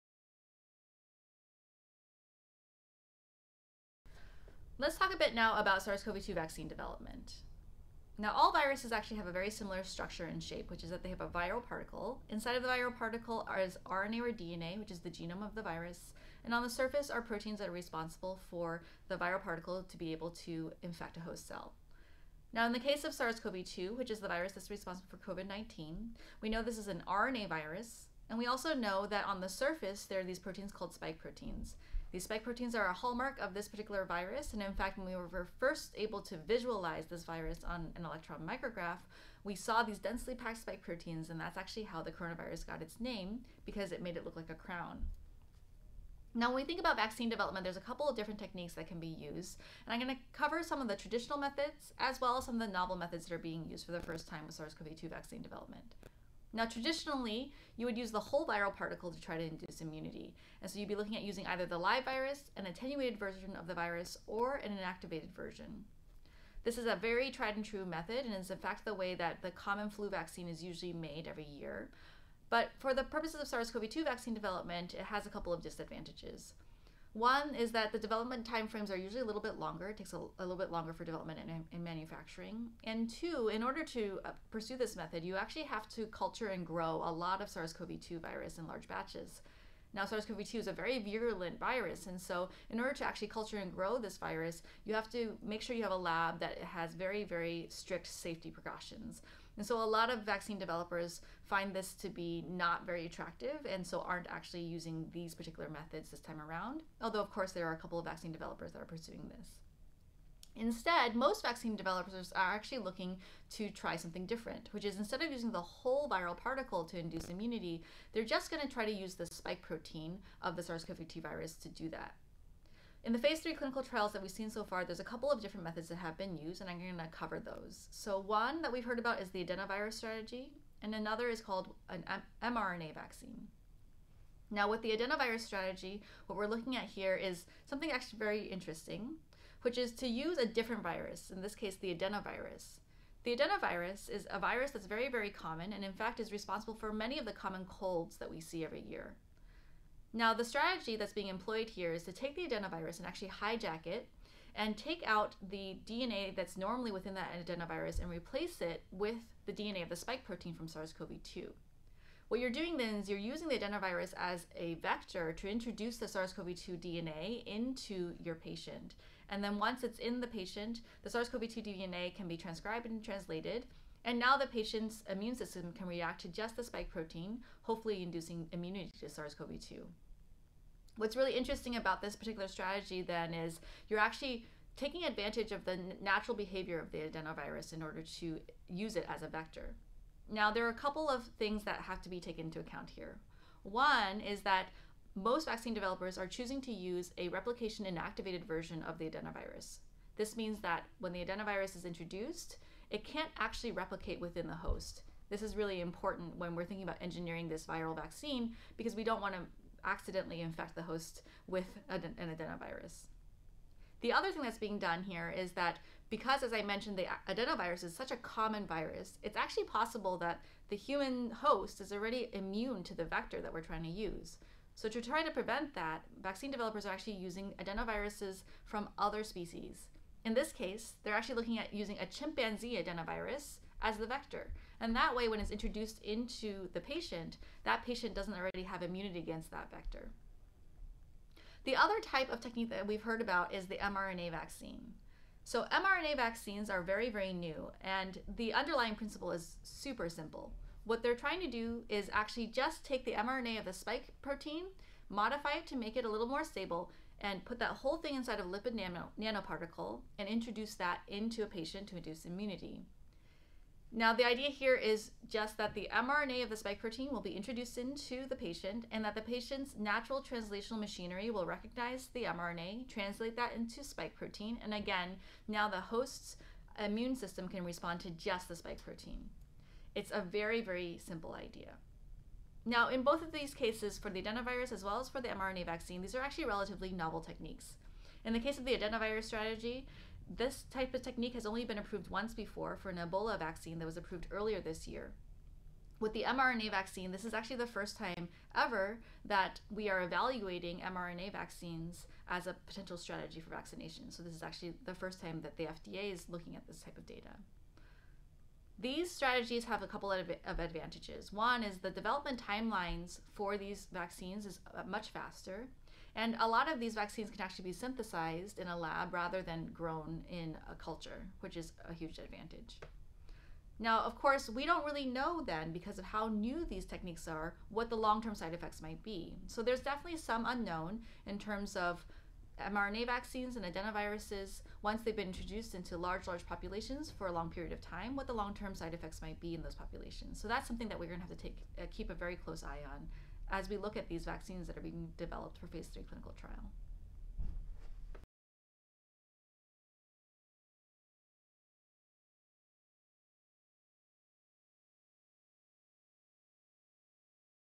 SARS-CoV-2 Vaccine Development
Learn more from our chief science officer, Alicia Zhou, Ph.D., on how vaccines are typically developed and how vaccines for SARS-CoV-2 are using novel methods.